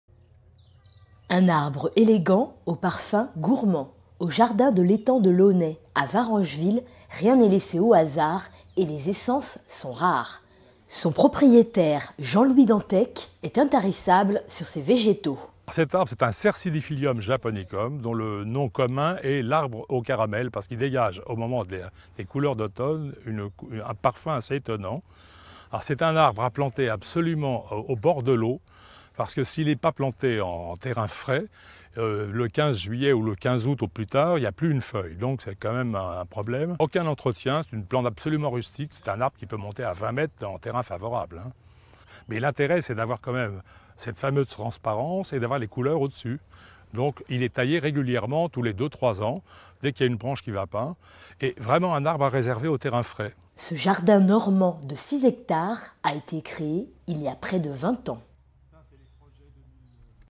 L'arbre au caramel : un camaïeu de jaune au parfum sucré
L’arbre au caramel (Cercidiphyllum japonicum) est un arbre au feuillage étonnant qui dégage une odeur de caramel en automne, lorsque ses feuilles brunissent.

Jean-Louis Dantec, au jardin de L'Étang de l'Aunay, à Varengeville, nous présente cet arbre à ford développement apprécie un terrain frais et humide.